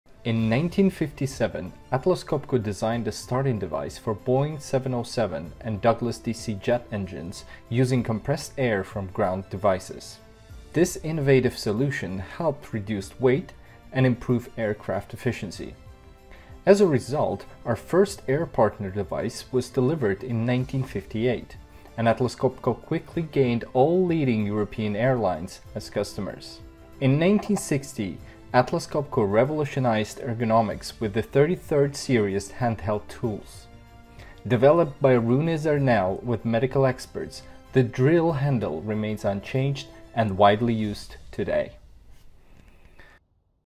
How does innovation drive growth at Atlas Copco Group
In 1957, Atlas Copco Group created a starting device that utilized small air turbines powered by compressed air to start the engines of the Boeing 707 and Douglas DC jet planes. We designed this starting device in less than a year and delivered the first Air Partner device to SAS in 1958. Later, major European airlines became our customers.
In 1960, Atlas Copco Group made significant progress in the field of ergonomic design by introducing the 33 series of handheld tools, which marked a true milestone in the history of ergonomic design. The series was developed through a collaborative effort between Atlas Copco engineer Rune Zernell and medical experts. To this day, the design of the drill handle remains largely unchanged and continues to be widely used.
Read about technological advances that have changed the world in the last 150 years